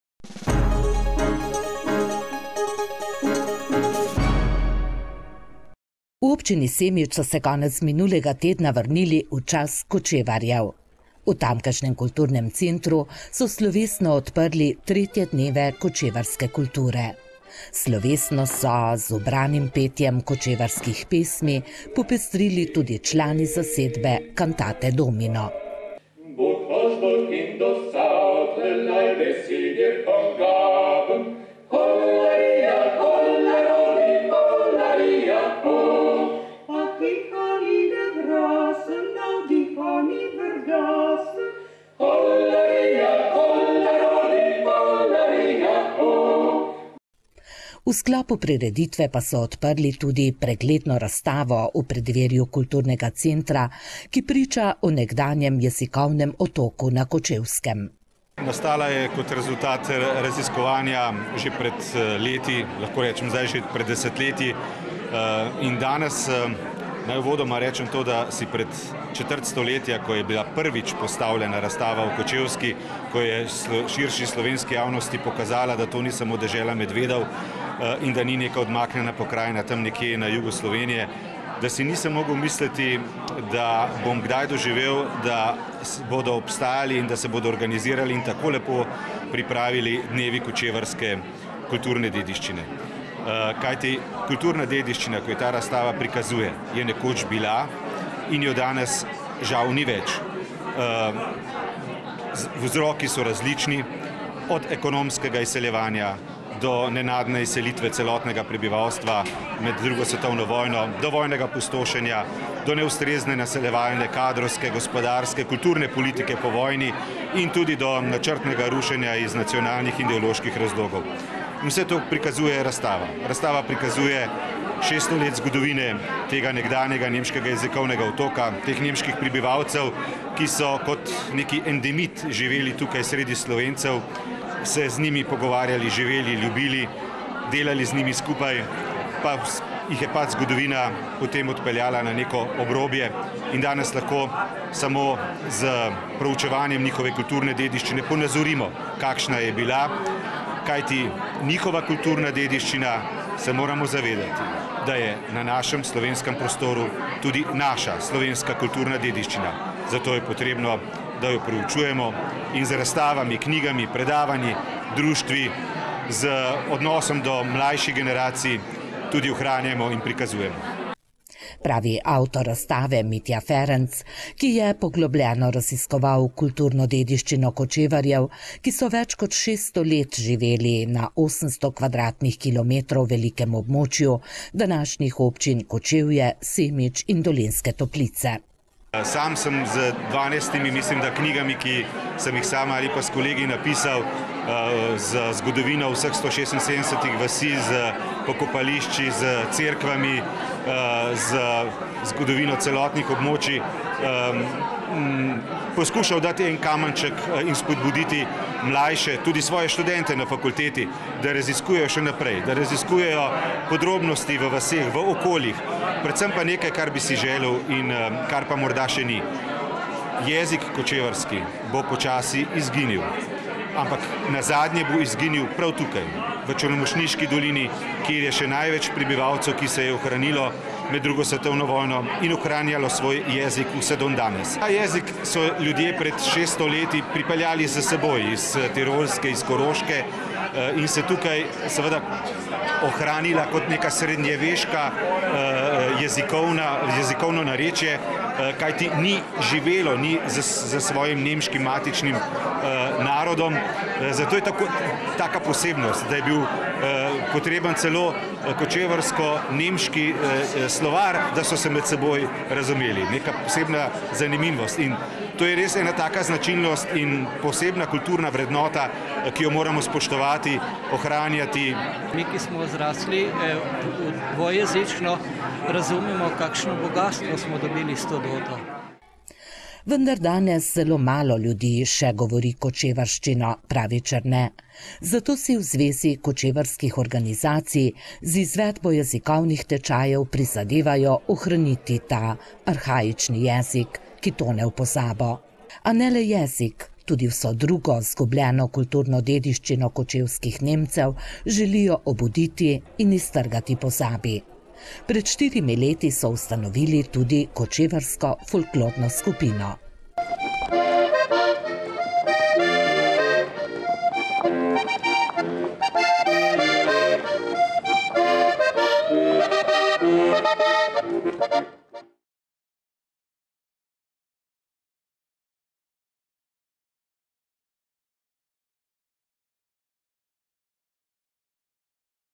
NEKDANJI NEMŠKI JEZIKOVNI OTOK NA KOČEVSKEM
SEMIČ - V občini Semič so se konec minulega tedna vrnili v čas Kočevarjev. V tamkajšnjem kulturnem centru so slovesno odprli 3. dneve kočevarske kulture. Slovesnost so s kočevarskimi pesmimi popestrili tudi člani zasedbe Canatate Domino.